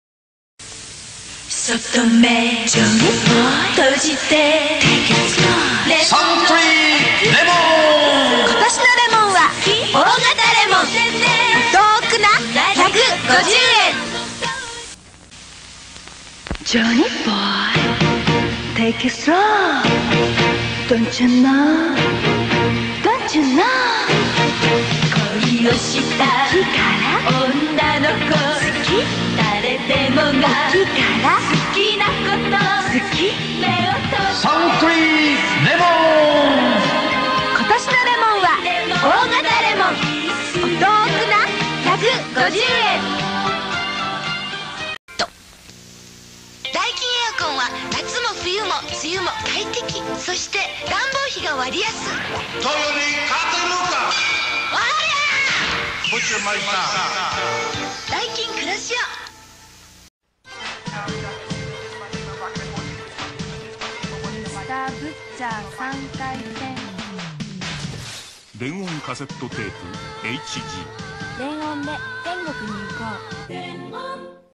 Abdullah the Butcher TV commercials （Japan）
アブドーラ・ザ・ブッチャー　CM集

1 Suntory Lemon（1980）
2 Suntory Lemon（1980）
3 Daikin Air Conditioner KUROSHIO（1983）
4 Denon Cassette Tape HG（1989）